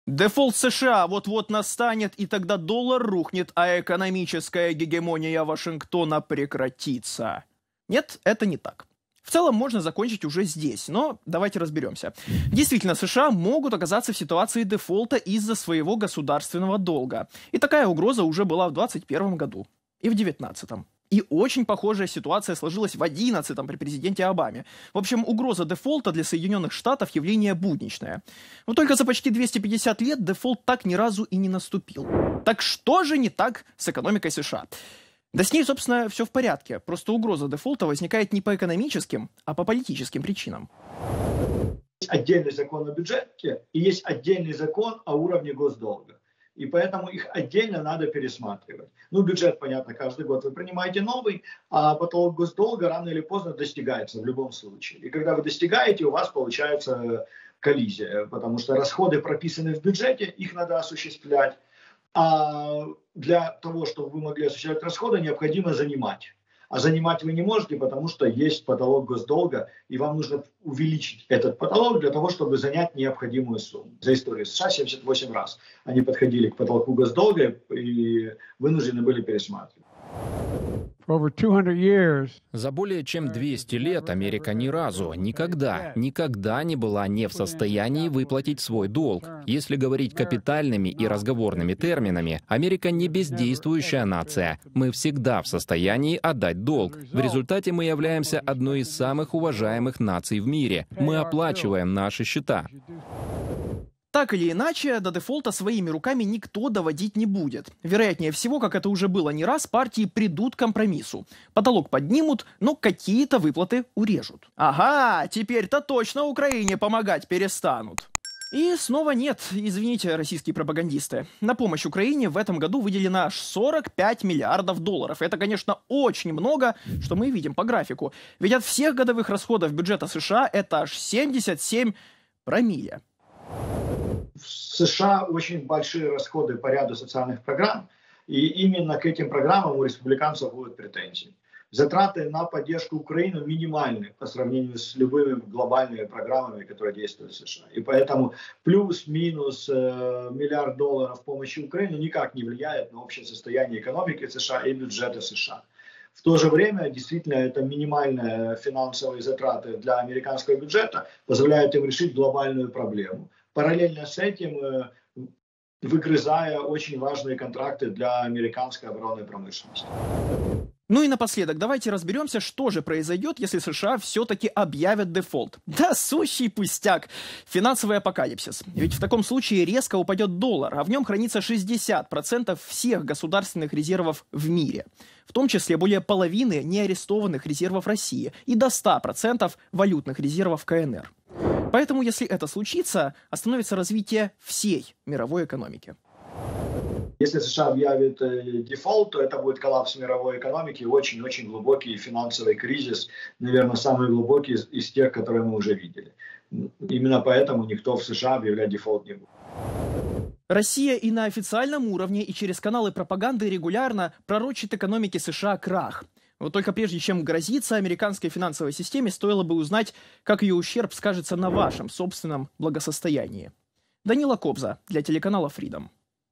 Реальное состояние экономики США. Разбор FREEДОМ
Президент Джо Байден сегодня проводит переговоры по вопросу госдолга США со спикером Палаты представителей, республиканцем Кевином Маккарти. Государственный долг достиг максимума, прописанного в законодательстве. Кремлевская пропаганда преподносит эту ситуацию как доказательство падения американской финансовой системы. Вот только на деле все обстоит совсем иначе. Подробности – в разборе FREEДОМ.